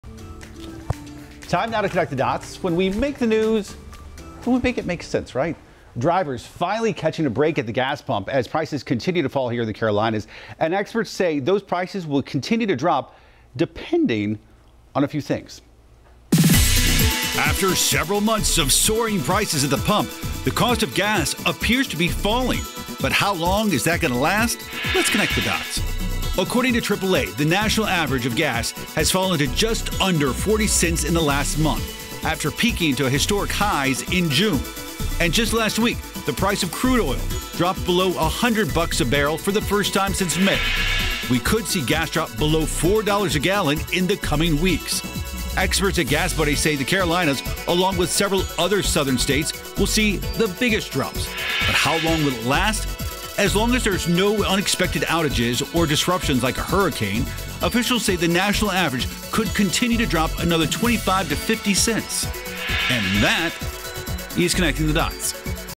Gas prices are finally going down. Will they stay below $4 a gallon?
Drivers are finally catching a break with falling gas prices in North Carolina and South Carolina. Experts say those gas prices could continue to drop across the United States, depending on a few things.

In this video, WCNC Charlotte's Ben Thompson connects the dots on the recent drop in gas prices and what's behind them.

After months of soaring gas prices, why are they going down now? AAA says the national average has fallen just under 40 cents in the past month after reaching historic highs earlier this year. In addition, the price of crude oil is below $100 a barrel for the first time in a while. This means we could see gas prices below $4 a gallon in the coming days and weeks.

Experts as GasBuddy say the Carolinas and other Southern states will see the biggest drops. How long will it last? Officials say as long as there isn't a long-term interruption in oil production, such as a major hurricane in the Gulf of Mexico, gas prices could drop by as much as 50 cents this fall.